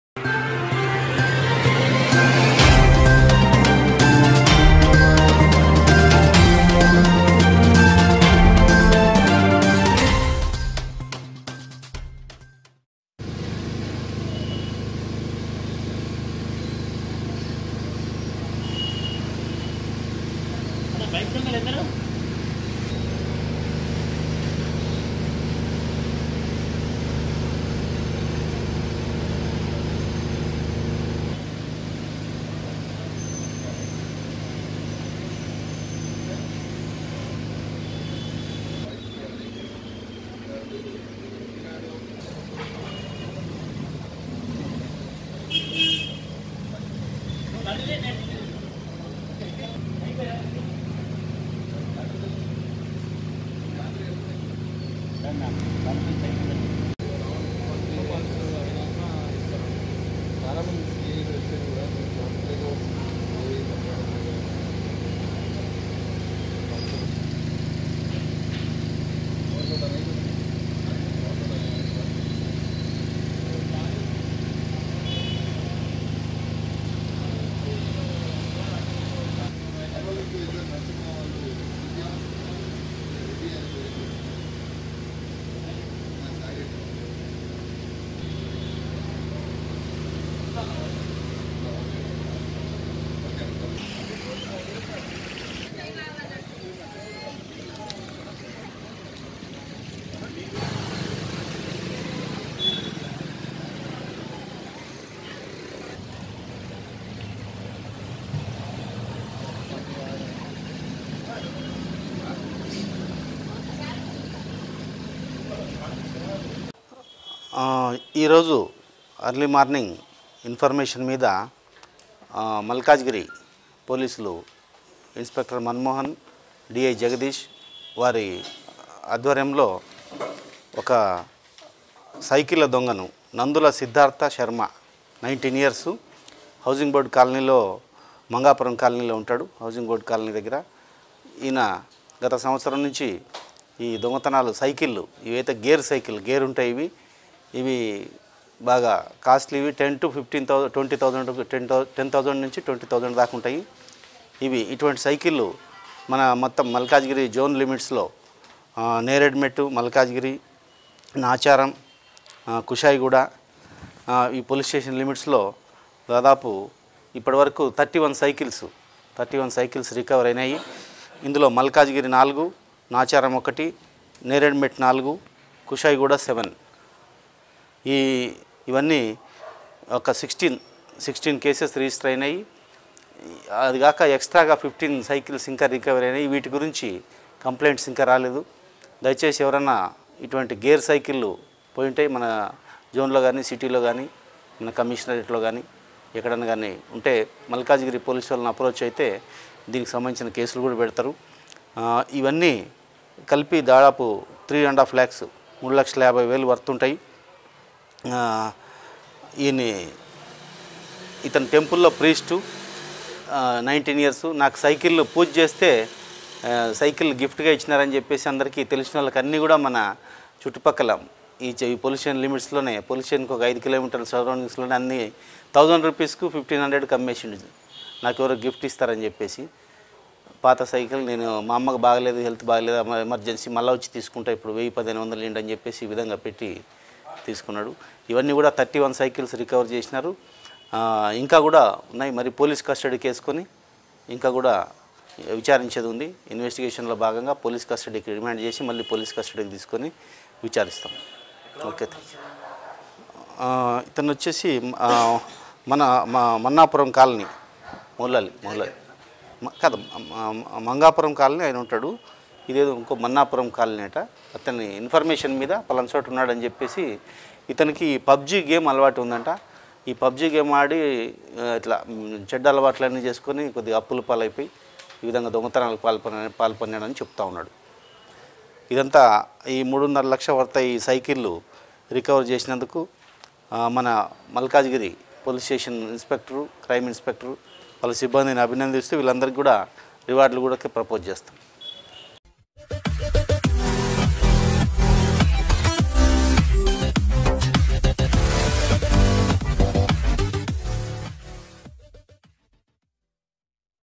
News53 I CHOR PUJARI KO MALKAJGIR POLICE NE KIYA GIRAFTAAR|MEDCHAL DIST|T.S| NANDULA SI
CHOR PUJARI KO MALKAJGIR POLICE NE KIYA GIRAFTAAR|MEDCHAL DIST|MALKAJGIR|T.S|

NANDULA SIDDHARTH SHARMA JO KE DIN ME MANDIR ME PUJARI AUR RAAT KE WAQT BACHON KI MEHNGI CYCLES CHORI'YAAN KARTA THA
AAJ POLICE NE EK ITTELA PAR ISEY GIRAFTAAR KIYA AUR ISKE QABZE SE 31 CYCLES BARAMAD KI GAYI JISKI LAG BHAG 3,50,000/- QEEMAT BATAYI GAYI HAI

AAYIE SUNTE HAI ACP MALKAJGIRI NARSIMHA REDDY KA BAYAN

Report By md Osman

FOR ANY NEWS/INFO/ADVTS